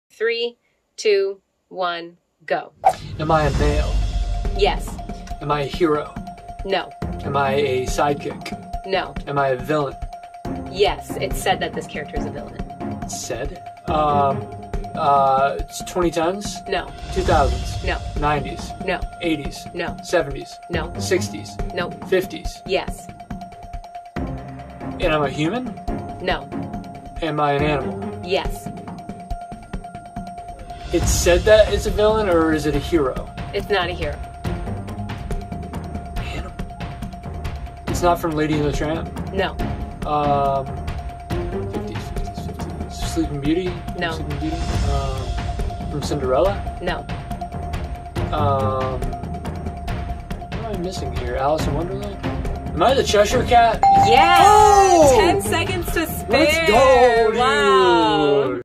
The chesire cat is kinda creepy 😺😮⏰️
Heads Up!
Nate tries to guess the Disney or Pixar character in less than 60 seconds! Can he do it? Find out in today's episode of SaraNate!